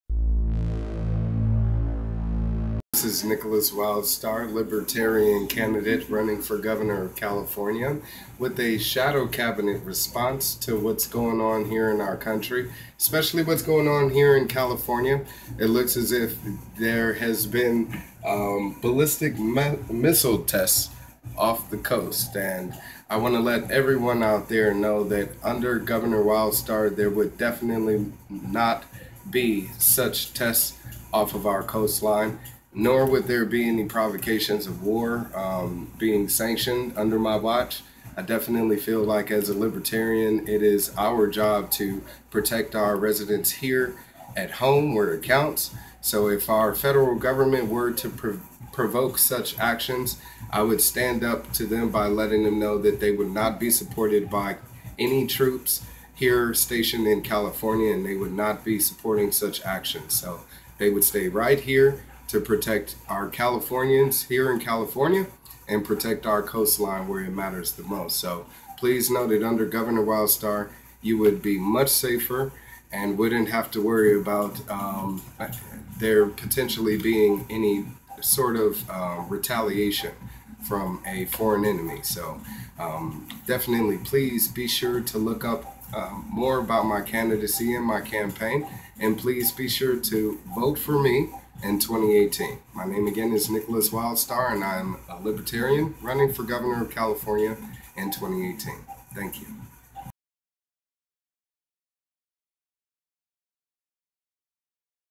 California Coast Ballistic Missile Test Response
Libertarian Candidate for California Governor makes 'shadow cabinet' response to recent actions by the Trump administration in coordination with Governor Jerry Brown to test ballistic missiles off the coast of California.

PLEASE support and join the rEVOLUtion!!!
NOW and follow Governor Wildstar on Facebook & Twitter!